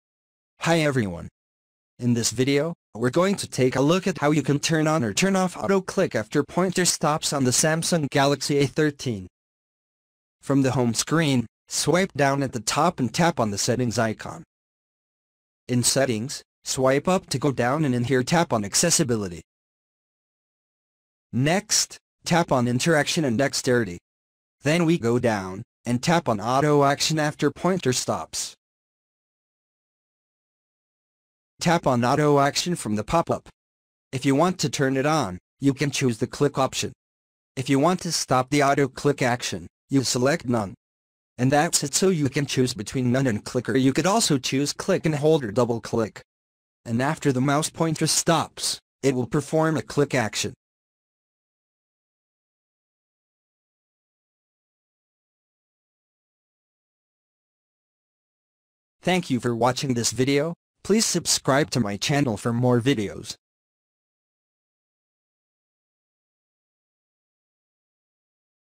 Samsung Galaxy A13: How to Turn On/Off Auto Click After Pointer Stops
Learn how you can Turn Of/Off Auto Click after pointer stops on the Samsung Galaxy A13.